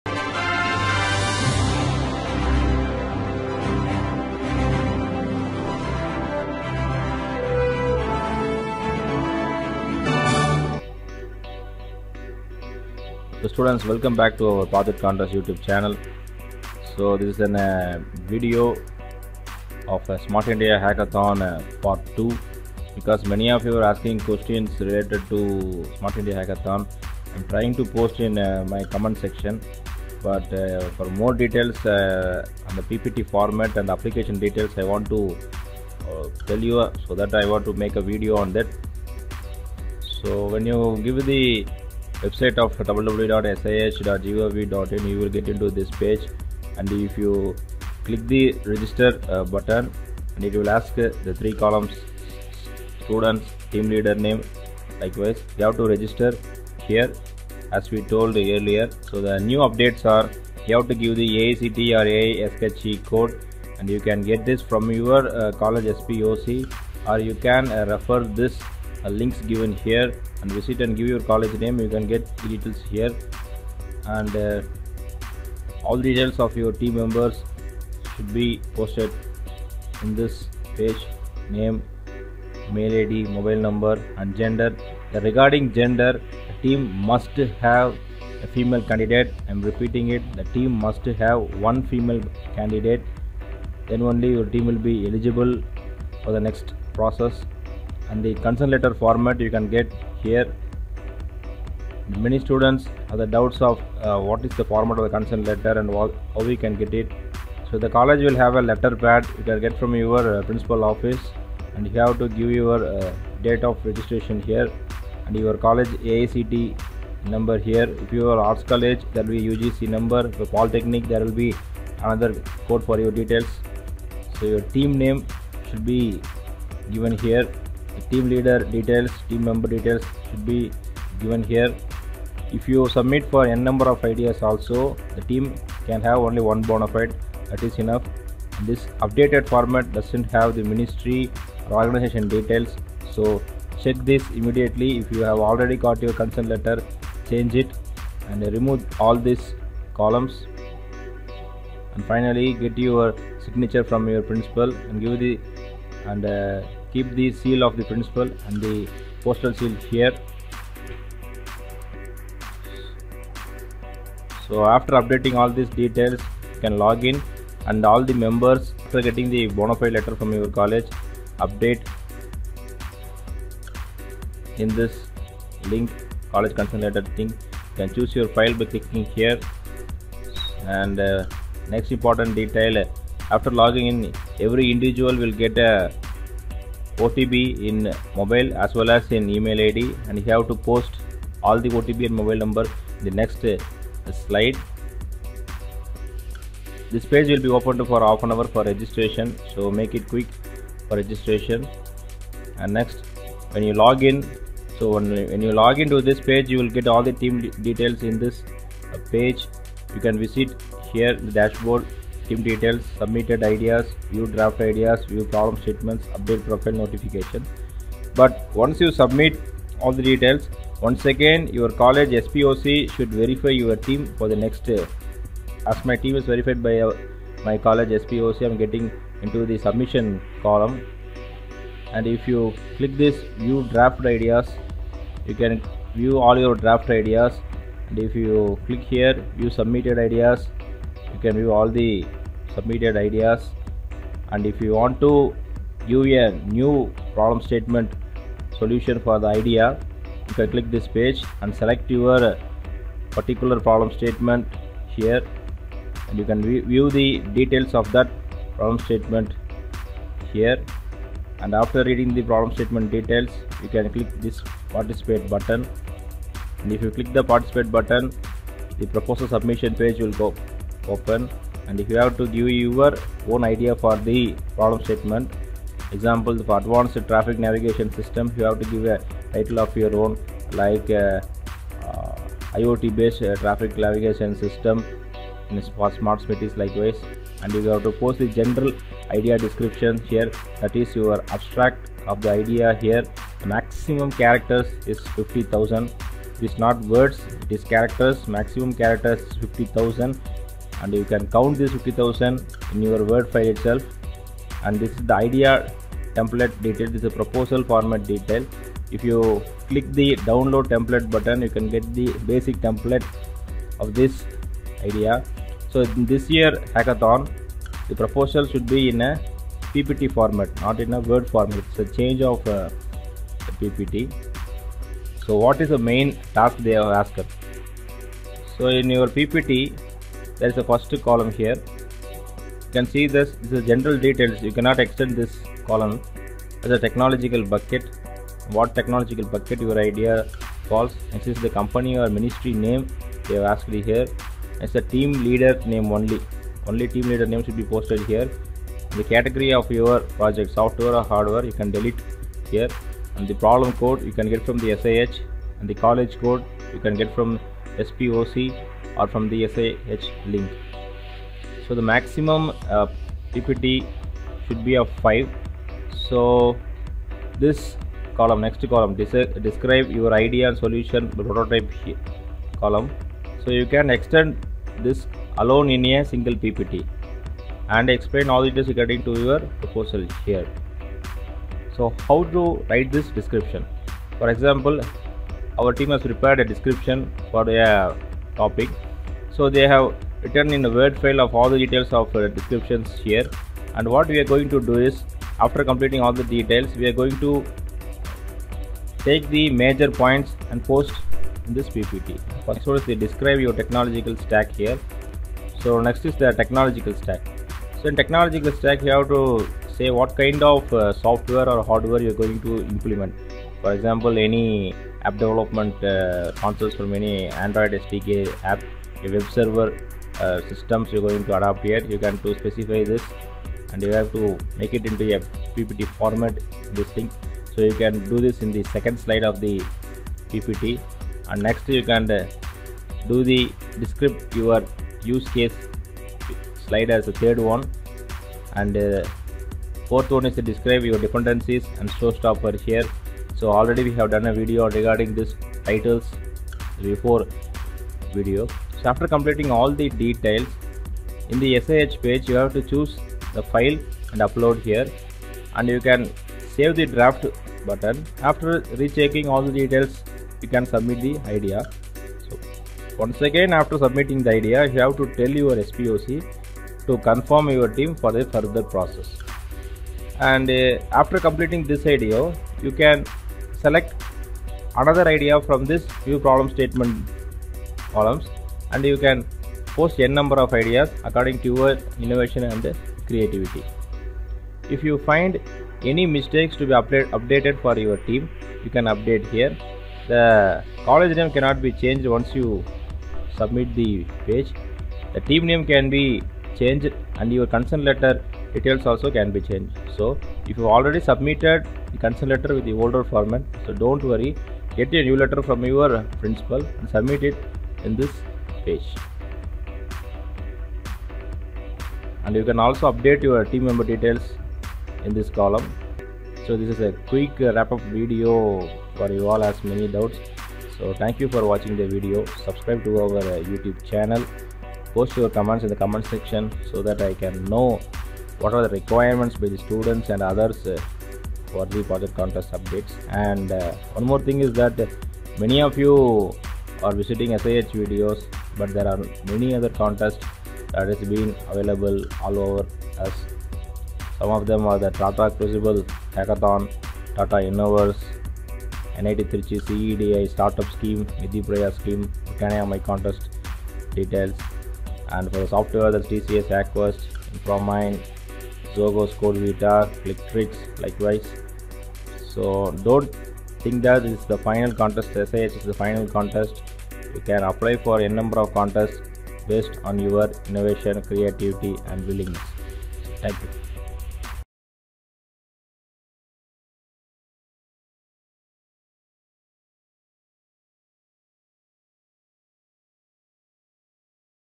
Sih ppt (2019) format I smart India hackathon-new info-part 2 I smart India hackathon
sih ppt format(smart India hackathon) 2019, consent letter updates, team member updates and more...